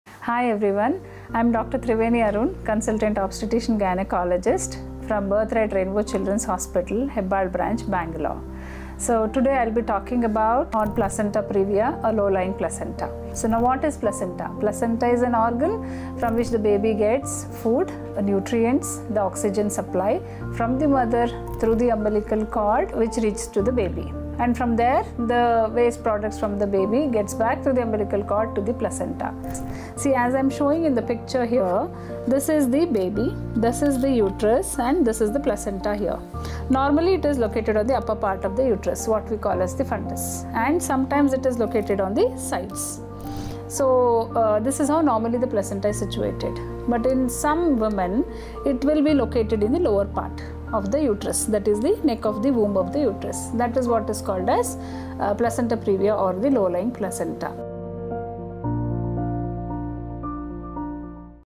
Placenta Previa
Placenta previa is a condition where the placenta is positioned lower than usual in the uterus, sometimes covering the birth passage.

In this video, Dr. Triveni Arun Akkiraju, Consultant -Obstetrics & Gynecology, Laparoscopy & High risk pregnancy, Hebbal, Bangalore, explains what placenta previa is in simple terms for expectant mothers.